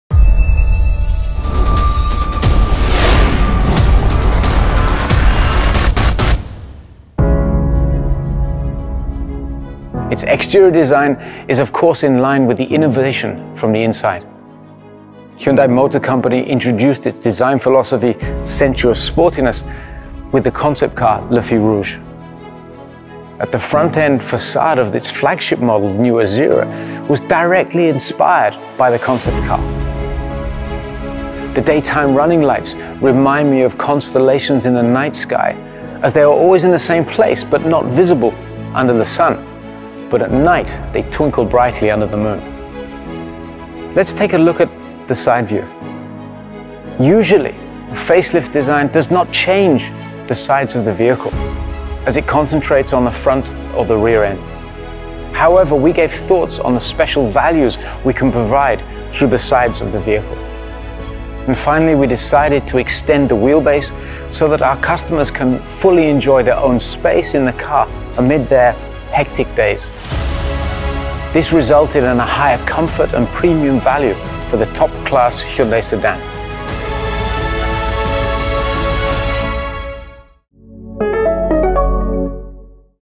The new Azera Exterior Design Film
استمتع برفاهية التصميم الداخلي مع الخطوط الرشيقة ومزيج من الألوان الأنيقة والملمس الناعم. هيونداي أزيرا الجديدة تصحبك راحتها إلى وجهتك لساعات طويلة من القيادة بمنتهى الهدوء والراحة.
Beauty that satisfies all senses. A seamless front-end architecture that boldly breaks the stereotype letting you be distinctive in a field of lookalikes. The modern radical visual of the new Azera inspires us not to be afraid of taking things to the next level.